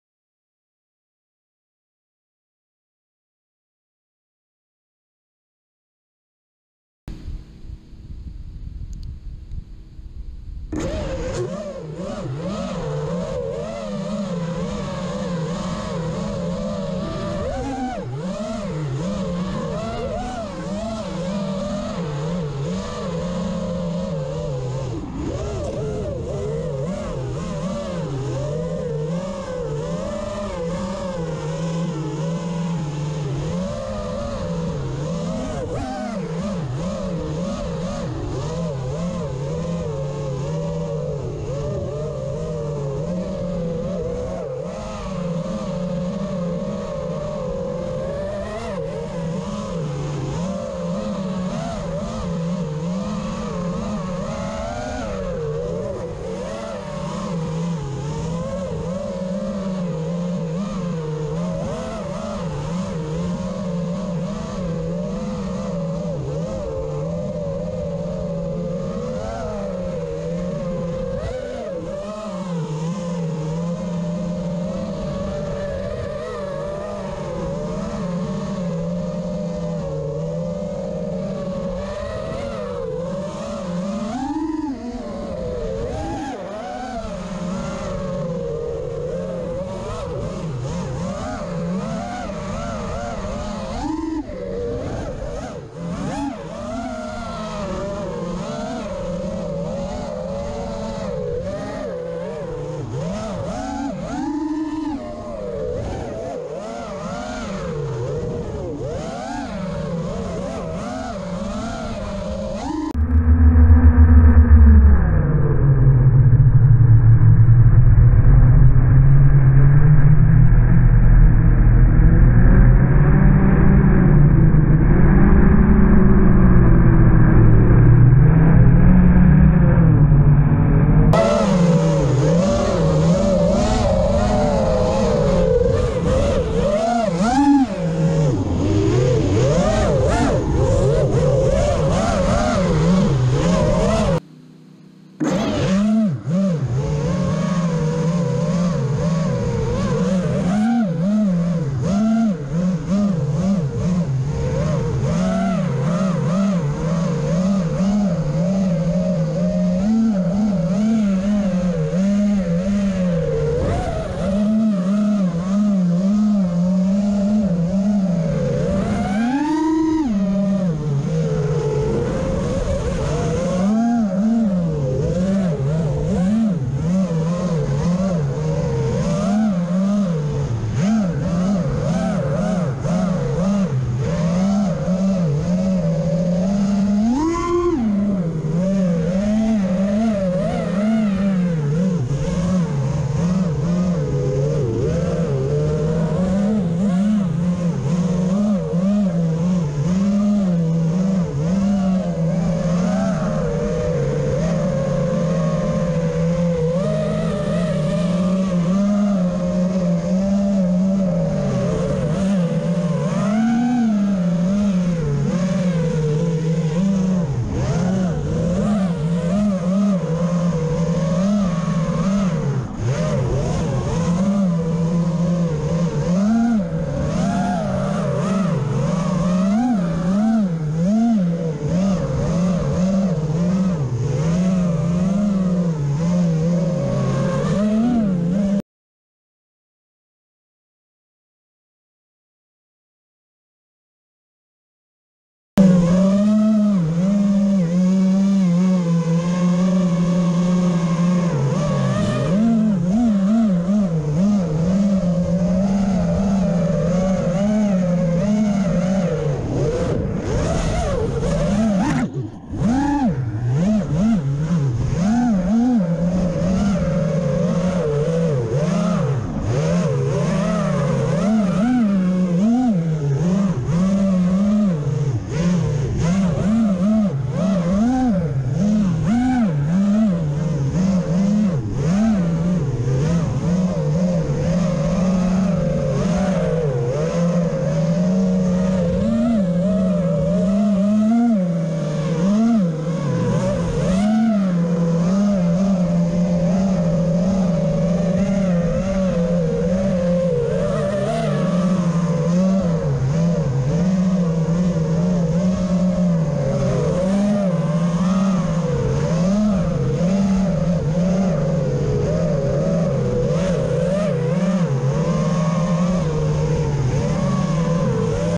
gopro session5
Ever since the runcam3 got wet,the battery life is very short.So I got a session5 waterproof,and I am trying out the different video settings.
Look at how low the water went down!